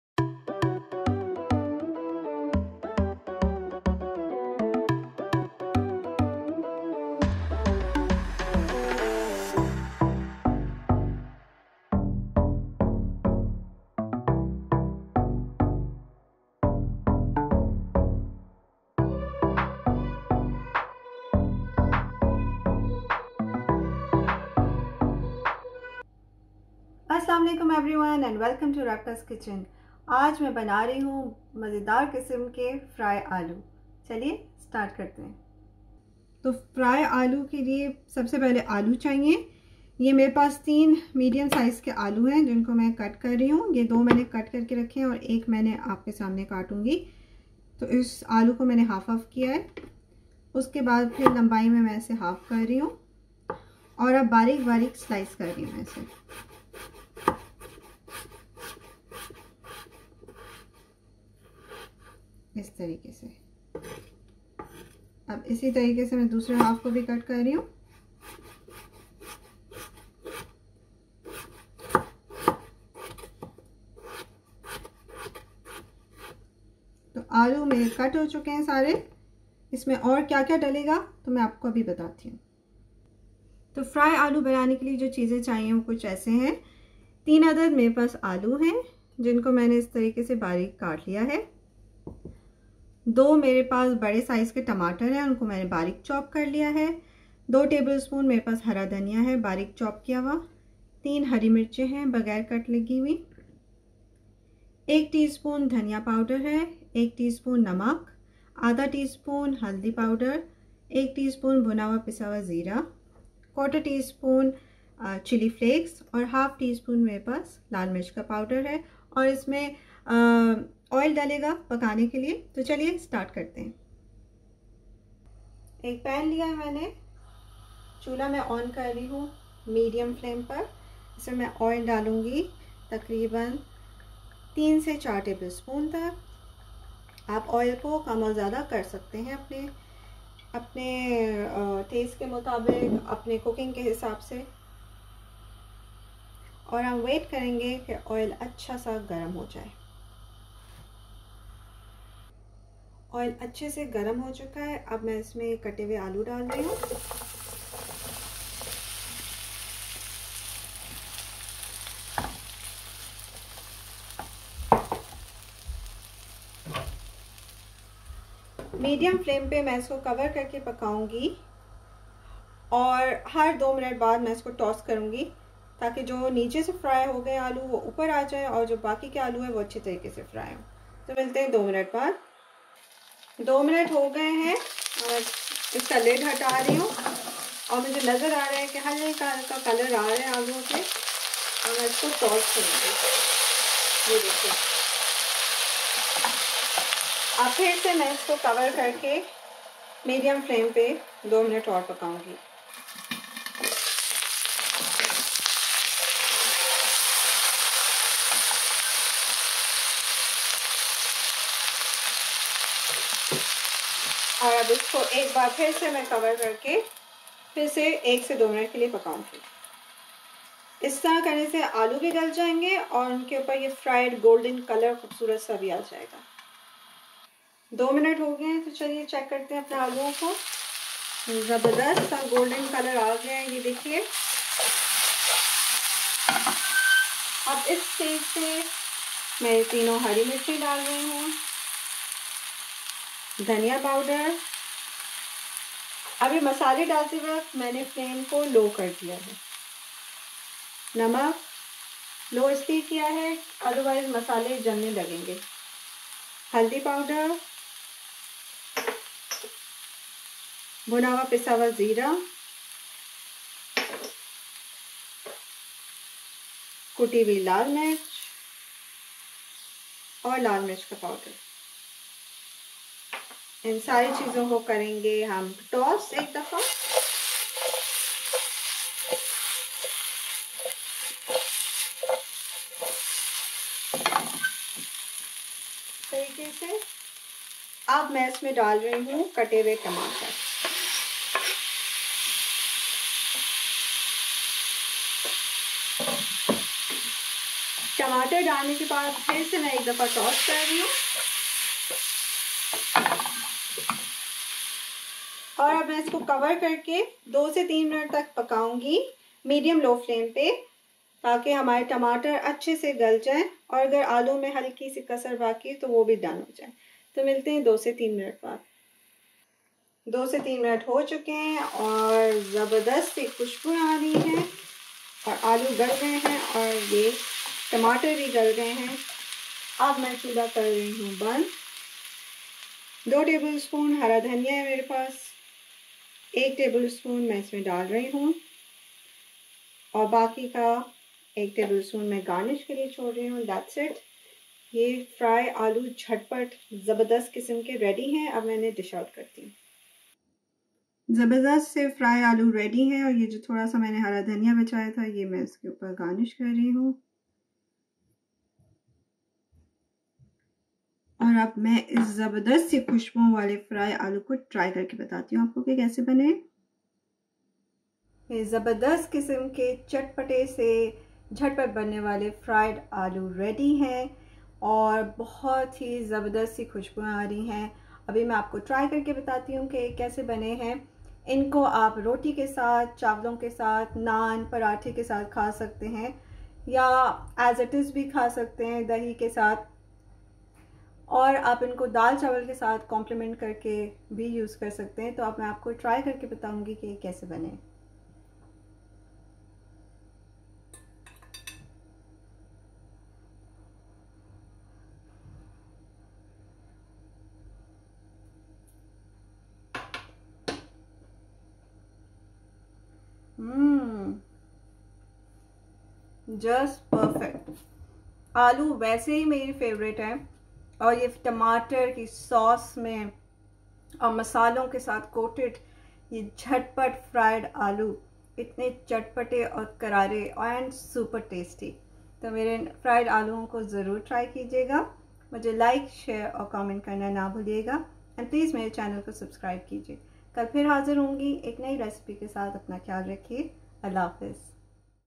Fried Potatoes with Tomato | Aloo Tamatar Fry Recipe in Urdu Hindi - Rebka's Kitchen
Tamatar Aloo Fry Recipe:
Ingredients:
• Aloo (potato) 3
• Tomatoes finely chopped 2
• Fresh Coriander chopped 2 tbs
• Green Chilli’s 3
• Roasted Zeera powder (Cumin powder) 1 tsp
• Lal mirch powder (Red chilli powder) ½ tsp or to taste